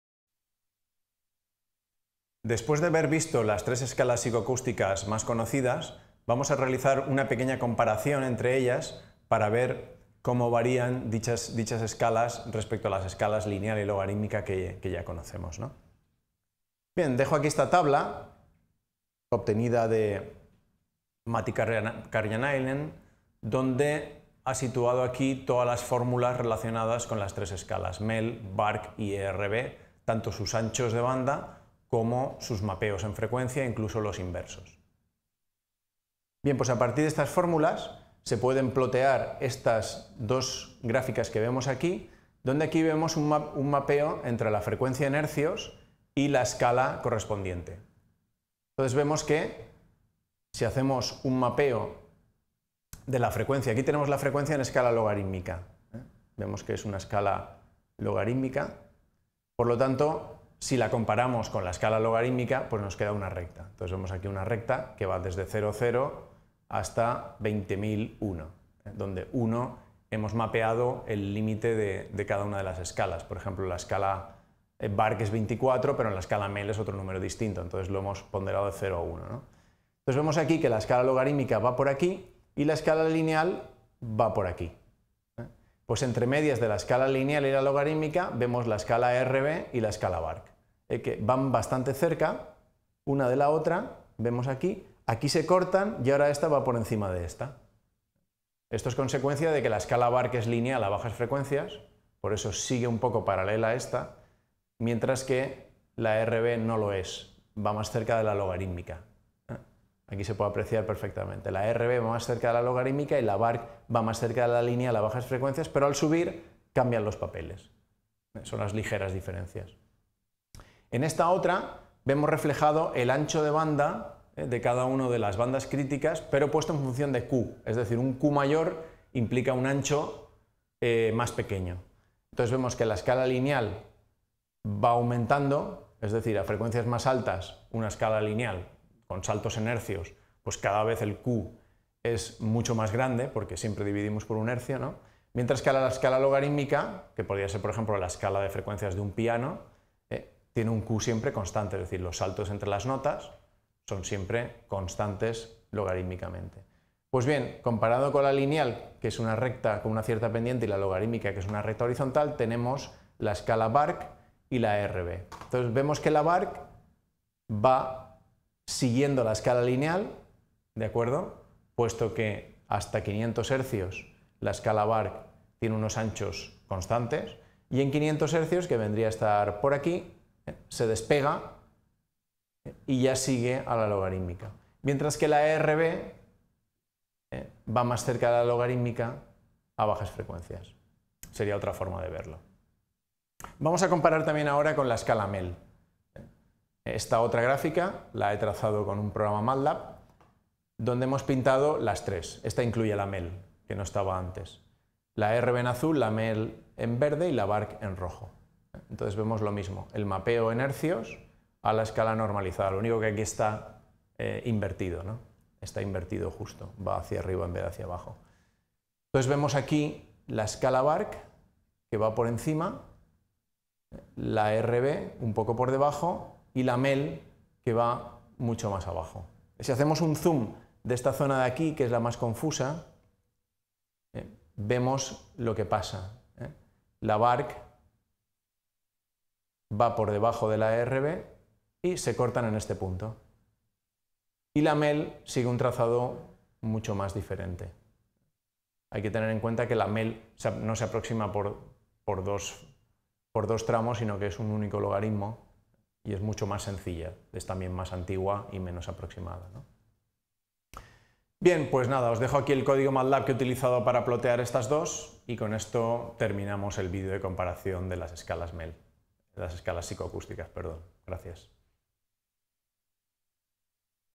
Comparación de escalas auditivas | 10/23 | UPV
Título: Comparación de escalas auditivas

Descripción automática: En este video se comparan las escalas psicoacústicas MEL, BARK y ERB con las escalas lineal y logarítmica tradicionales. Se menciona una tabla que contiene las fórmulas de las escalas, incluyendo los mapeos de frecuencias y los anchos de banda. Se muestran gráficos donde se plotean los mapeos de frecuencia en Hertz a cada escala psicoacústica, así como sus relaciones con las escalas lineal y logarítmica.

Se observa que la escala BARK se alinea con la escala lineal a bajas frecuencias pero tiende hacia la logarítmica a frecuencias más altas. En contraste, la escala ERB se aproxima más a la logarítmica incluso en bajas frecuencias. Además, se discute cómo el ancho de banda crítico varía con la frecuencia en cada escala, utilizando como referencia el factor Q, que implica que un Q mayor corresponde a un ancho de banda más estrecho.

Finalmente, se compara la escala MEL con las otras dos, notando que posee un comportamiento diferenciado, ya que se basa en un único logaritmo y es más antigua, resultando en una aproximación menos precisa de las sensaciones auditivas. Se cierra el video compartiendo el código usado para generar los gráficos y concluyendo la explicación sobre las escalas psicoacústicas.

Autor/a: López Monfort José Javier

Curso: Este vídeo es el 10/23 del curso MOOC Codificación de audio: Más allá del MP3.